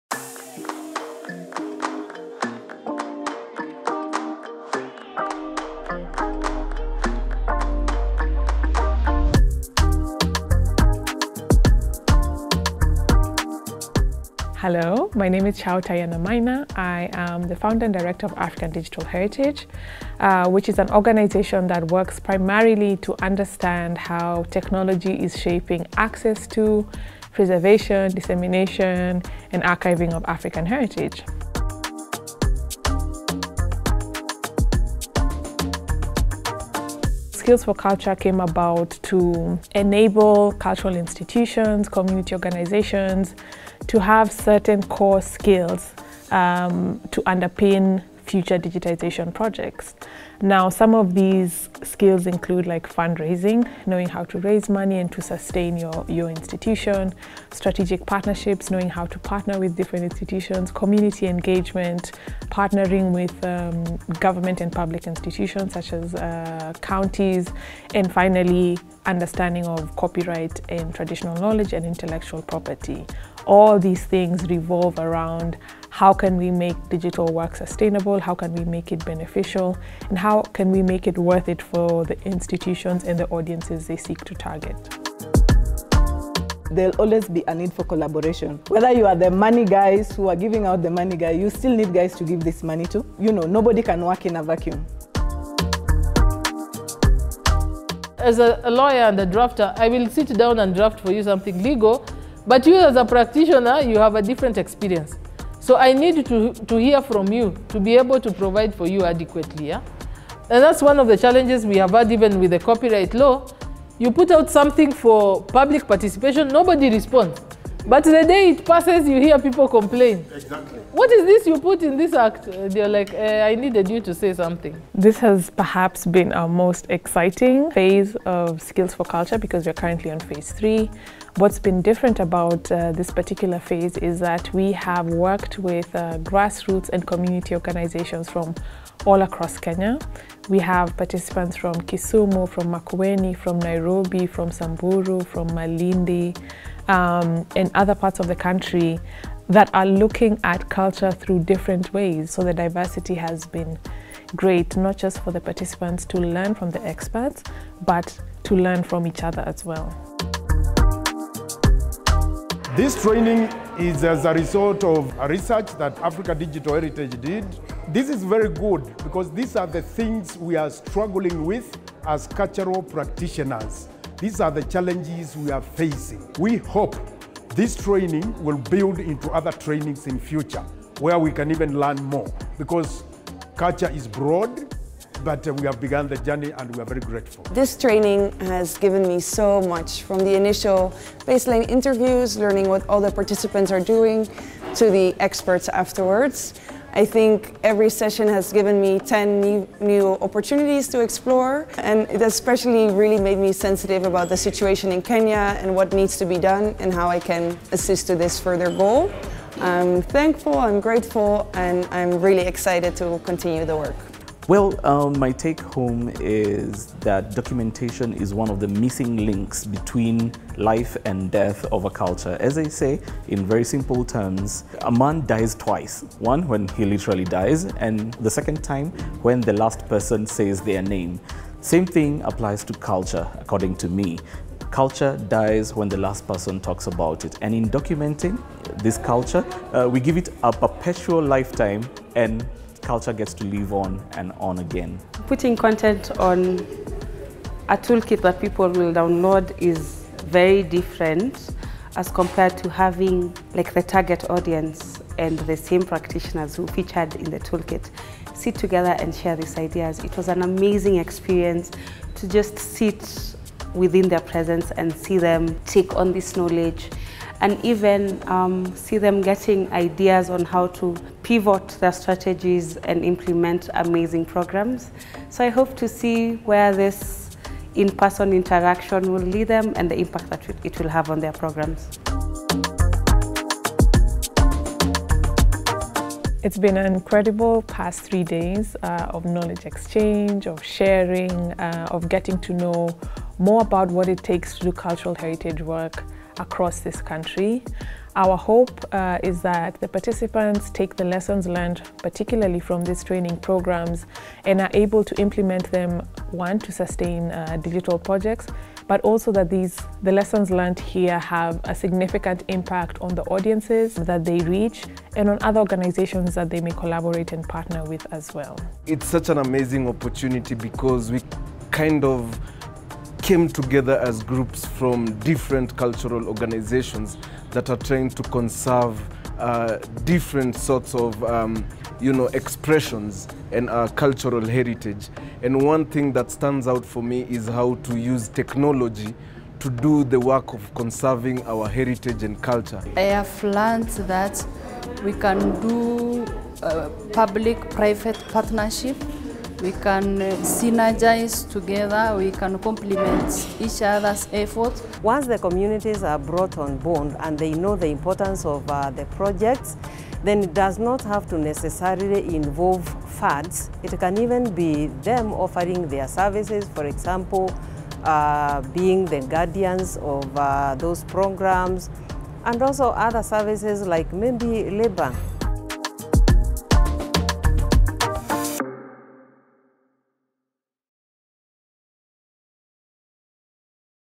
Highlights from the African Digital Heritage Skills for Culture Training And Mentorship Program
This year, African Digital Heritage tailored educational training opportunities and materials to the contexts, collections and needs of specific community-based cultural heritage programs and organisations. This video documents the experience of the participating organisations and practitionners during the 2023 three-day Skills for Culture training and mentorship program.

Skills for Culture is an African Digital Heritage program conceptualised in 2019 as part of the British Council's Cultural Heritage For Inclusive Growth program. It seeks to increase the visibility, ownership, accessibility, inclusivity, and transmission of cultural heritage by bolstering the status of cultural heritage and translating it for the next generation through contemporary practice, participation, and technology.

To access and download the specially curated toolkit used to guide and facilitate the trainings,